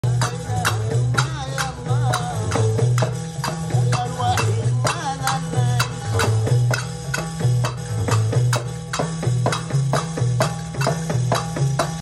Marrakech vibes Gnawa music 🎶
Gnawa music is a rich Moroccan force of ancient African Islamic spiritual religious songs and measures. Its well- saved heritage combines ritual poetry with traditional music and dancing. The music is performed at lila, entire collaborative nights of festivity devoted to prayer and mending guided by the Gnawa maalem, or master musician, and their group of musicians and hop. Though numerous of the influences that formed this music can be traced to sub-Saharan West- Africa, its traditional practice is concentrated in Morocco.